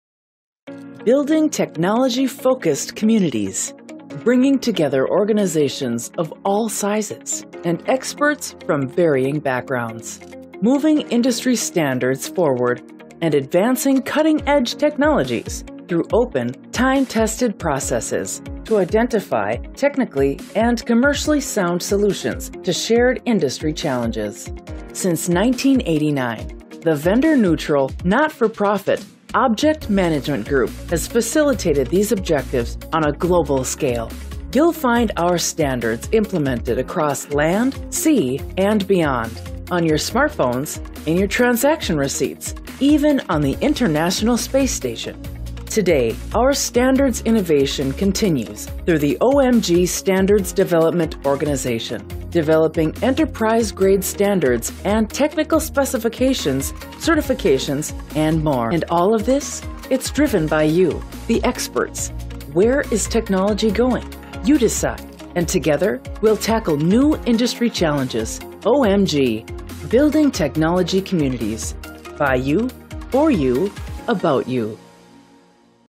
OMG Standards Development Organization Overview
The Object Management Group® Standards Development Organization (OMG® SDO) is an international (25+ countries), membership-driven (220+ organizations) and not-for-profit consortium. Since our founding in 1989, we have published over 260+ technical standards by providing a neutral forum where researchers, vendors, and end users in academia, government and industry collaborate as equals. Our community gathers the best practices from their fields to generate specifications and standards that drive the adoption and innovation of cutting-edge technology spanning industries worldwide. We are a community of/with standards!